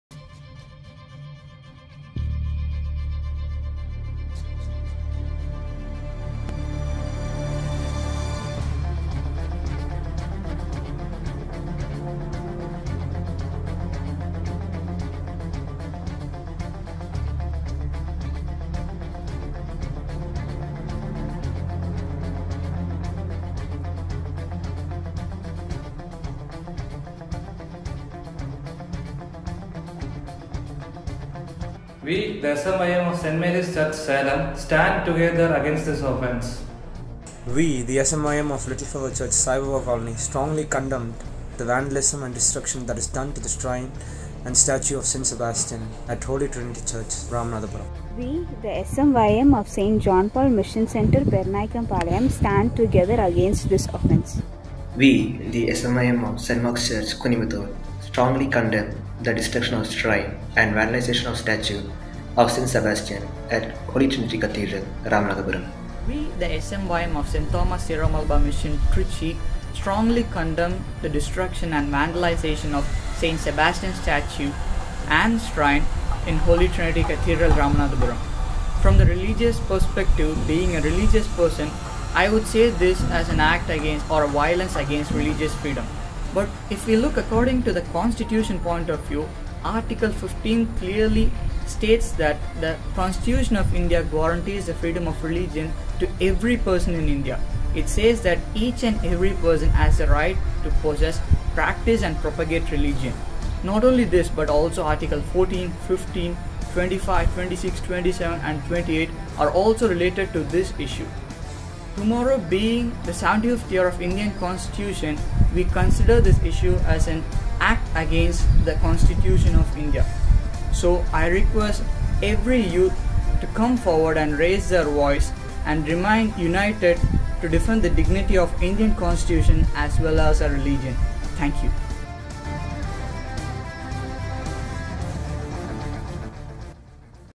Virtual Protest | SMYM Diocese of Ramanathapuram
We the SMYM of Diocese of Ramanathapuram strongly condemn the destruction and vandalization of shrine and statue of Saint Sebastian at Holy Trinity Cathedral, Ramanathapuram.